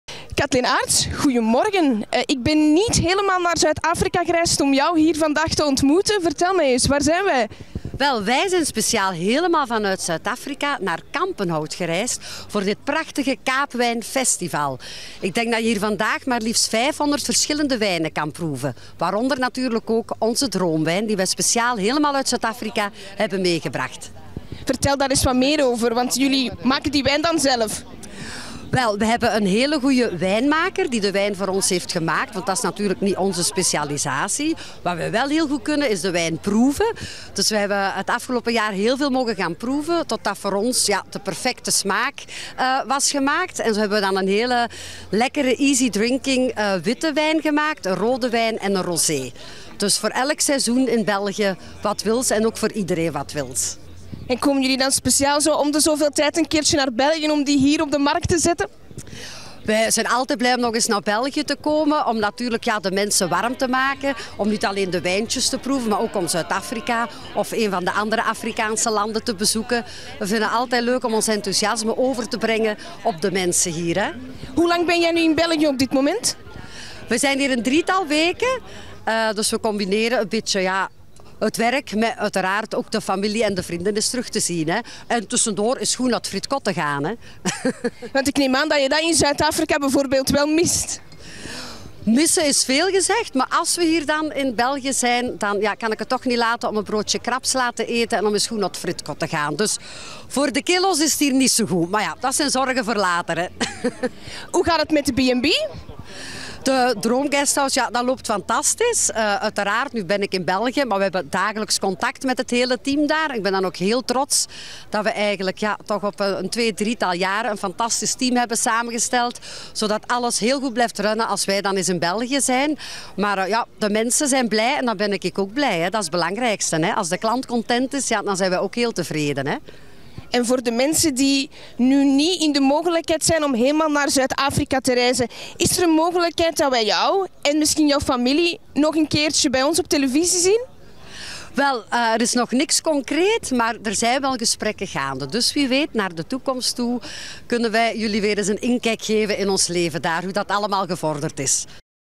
Is Kathleen Aerts in België voor de reünie van de originele K3?
Kathleen Aerts kwam met het hele gezin deze zomer voor drie weken terug naar Vlaanderen. Naast een goed pak friet en toastjes krabsla kwam het gezin ook nog voor andere doeleinden terug naar hun heimat.

Dat Kathleen en Steven in Zuid-Afrika Guesthouse 'Droom' hadden geopend, kon je volgen in 'Villa Zuid-Afrika' op VTM 2, maar het koppel heeft sindsdien niet stilgezeten. Sinds kort is ook de 'Droom'-wijn op de markt en die kwamen Kathleen, Steven en de kinderen maar al te graag presenteren op het Kaapwijnfestival in Kampenhout.